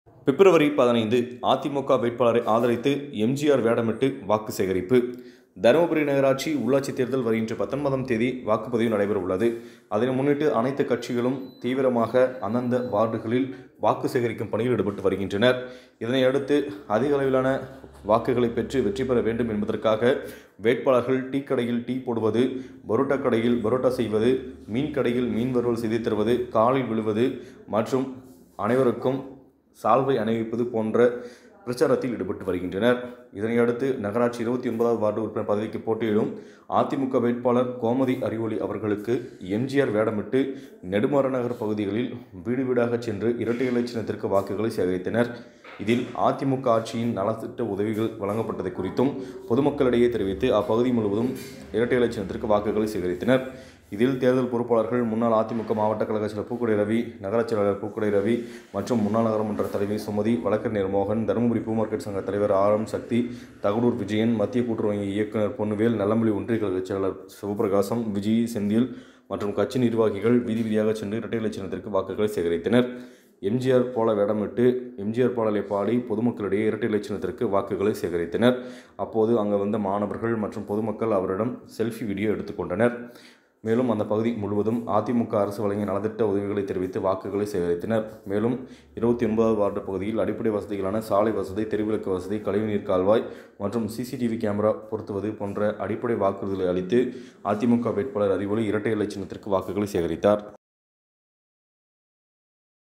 புரட்சித்தலைவர் எம்ஜிஆர் வேடம் அணிந்து இரட்டை இலை சின்னத்திற்கு வாக்கு சேகரிப்பு
Video from Periyasamy